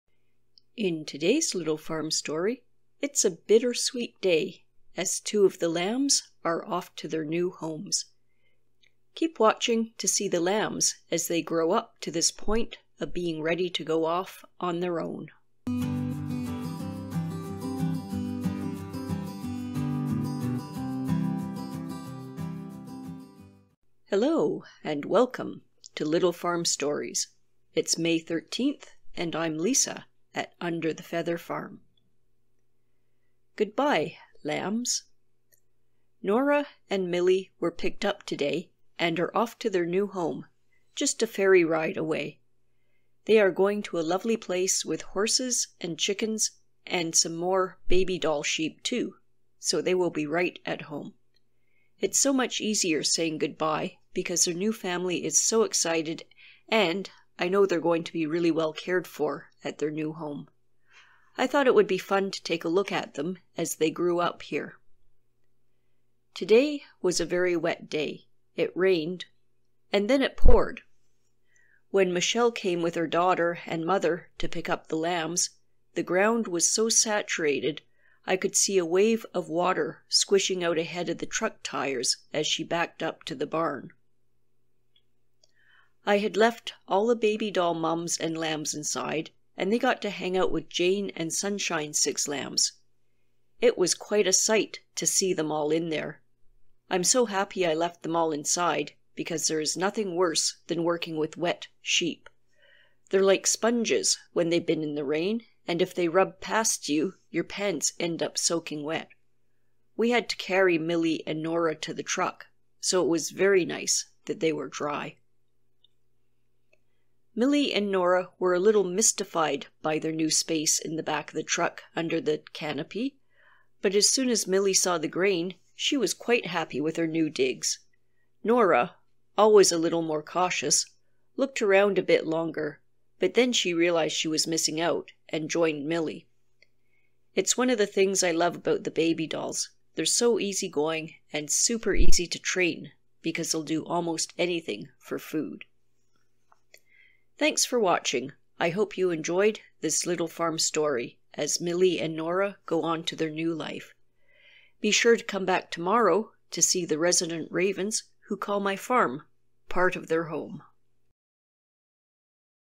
The LAMBS Are Off To Their NEW HOME - May 13 Little Farm Stories
In today's Little Farm Story we say goodbye to Millie and Nora as they go to their new home on a very wet and rainy day.

Follow the gentle farm life at my small farm in south western British Columbia in these Little Farm Stories.